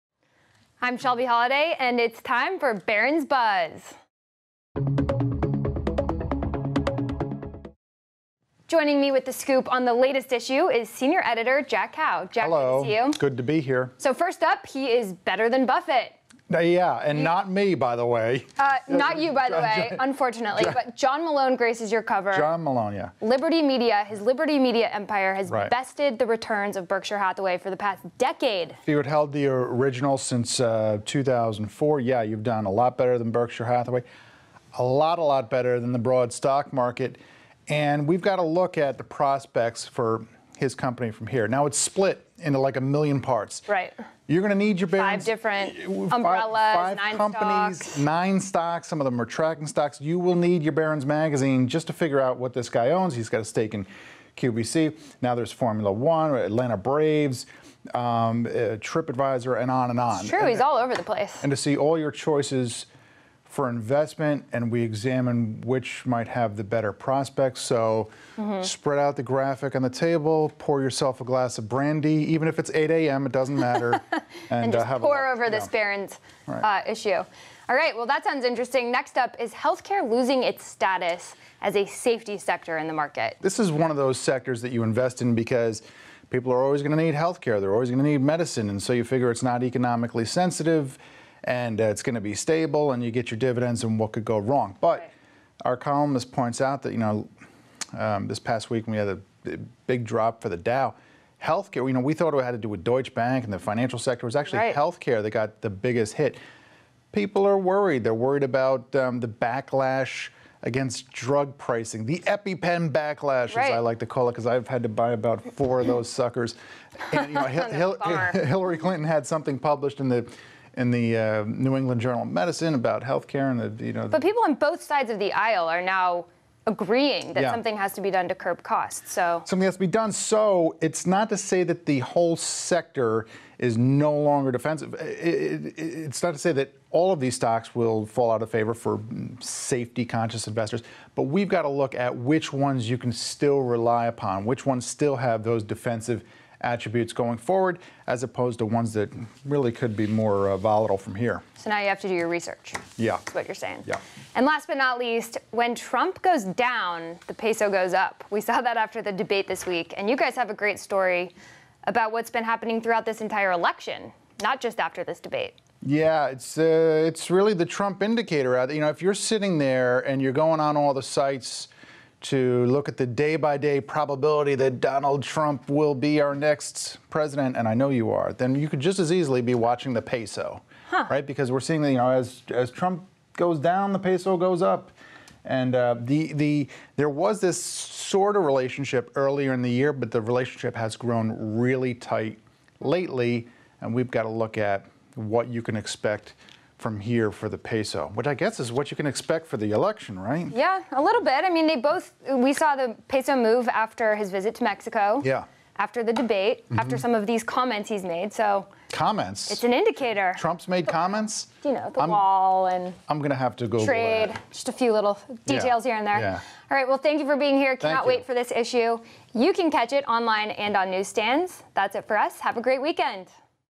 Barron's Buzz: John Malone Better Than Buffett?
Barron's senior editor Jack Hough and WSJ's Shelby Holliday preview the new issue of Barron's. Topics include the investment strategies of Liberty Media chairman John Malone. Plus, have heath care stocks lost their luster? Also, how Donald Trump's comments impact the Mexican Peso.